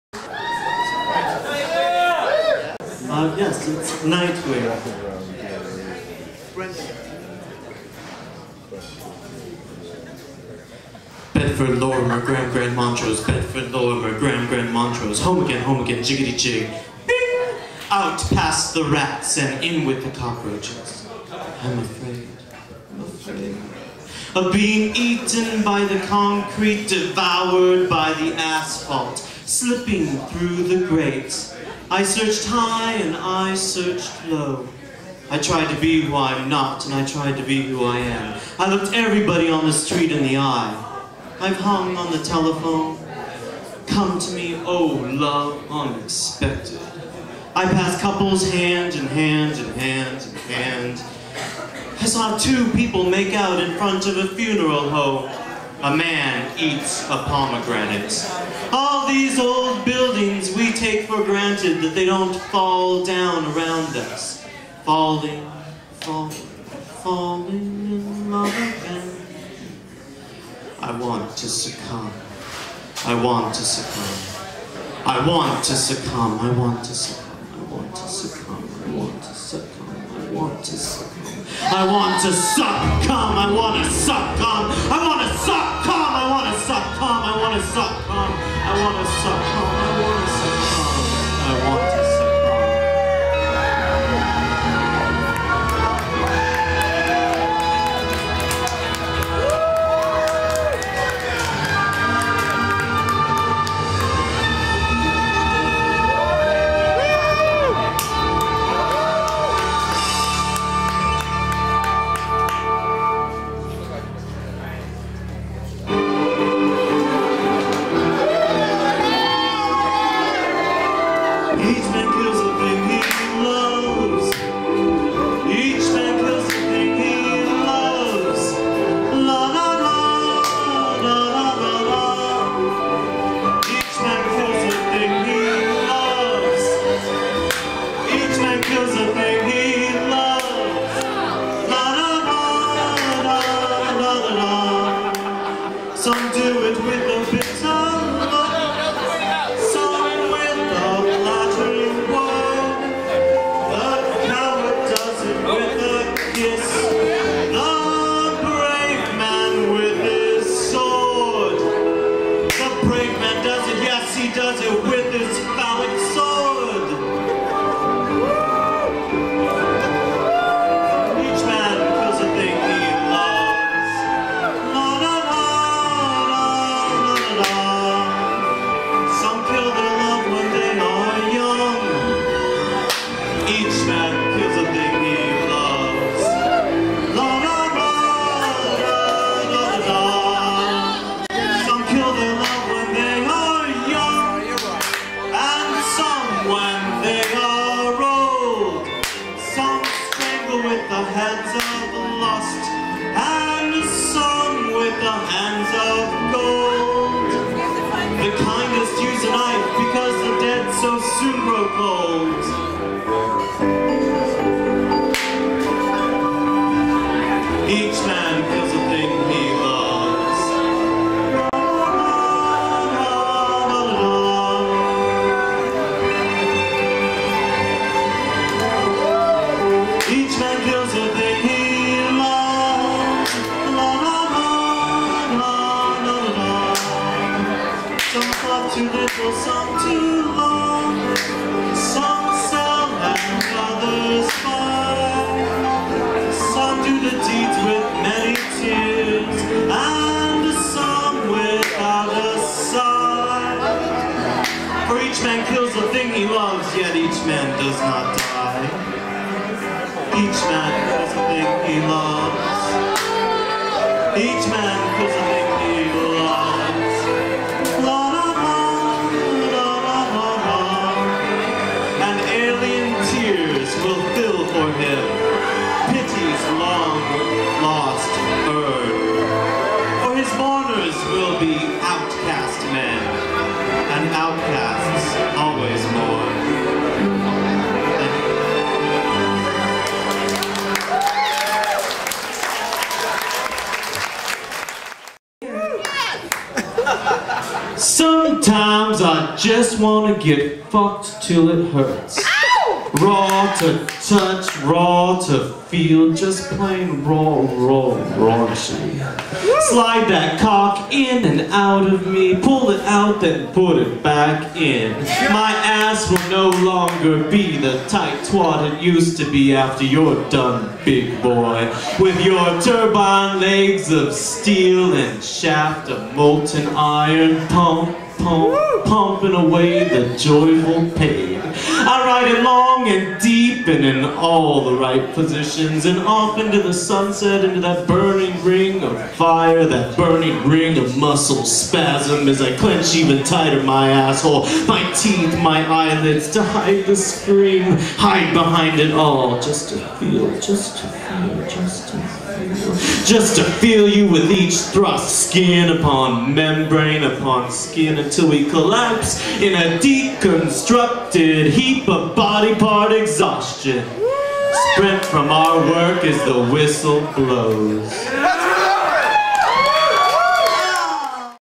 philip trevino- joe's pub performance
performance artist, philip trevino, performs at joe's pub.